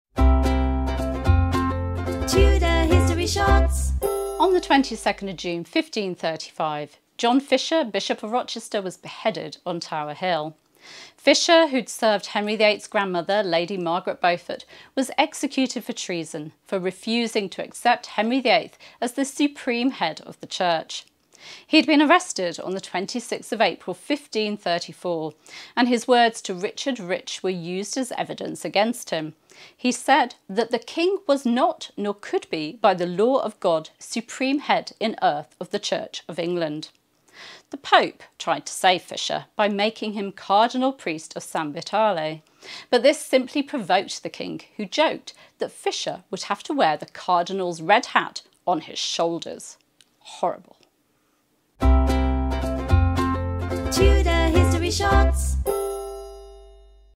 22 June - The execution of Bishop Fisher
On 22nd June 1535, in the reign of King Henry VIII, John Fisher, Bishop of Rochester, was executed. Why was Fisher executed and how did the pope try to save him?

Find out in this edition of TudorHistoryShorts from historian Claire Ridgway.

Claire has also done an online history course, The Life of Anne Boleyn, for MedievalCourses.com